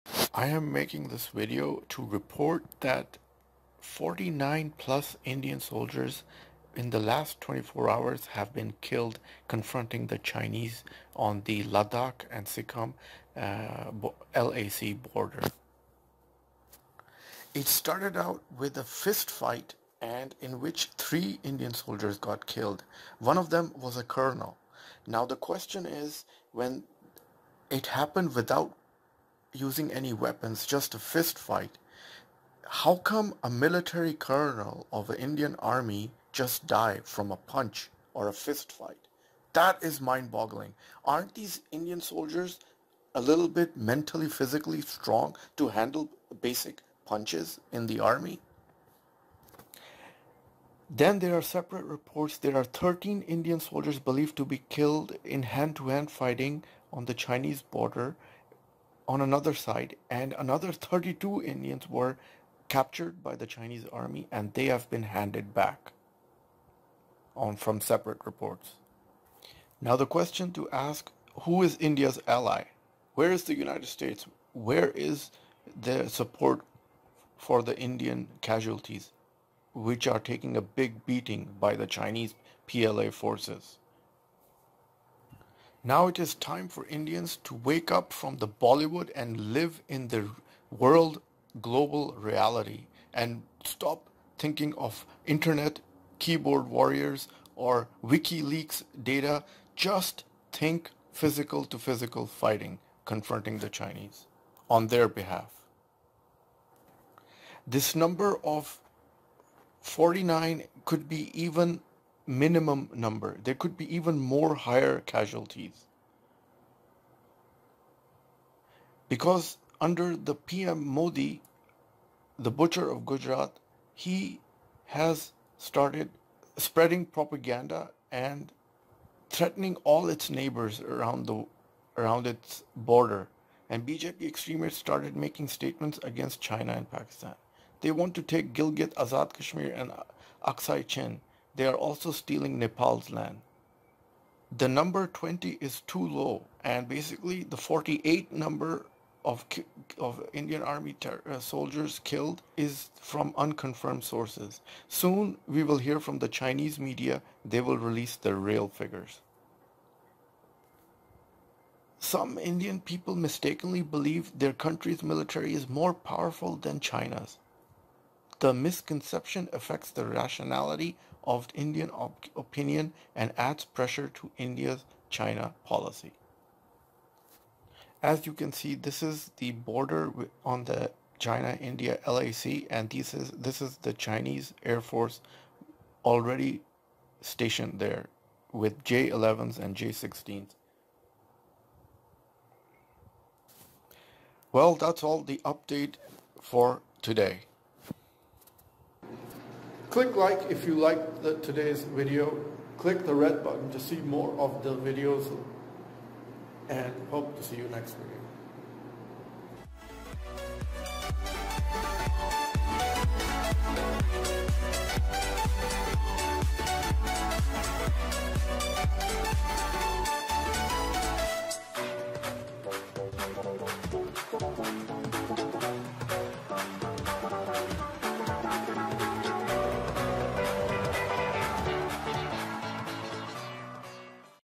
49 Indian soldiers killed by Chinese PLA in Ladhak
49 Indian soldiers killed by Chinese PLA in Ladhak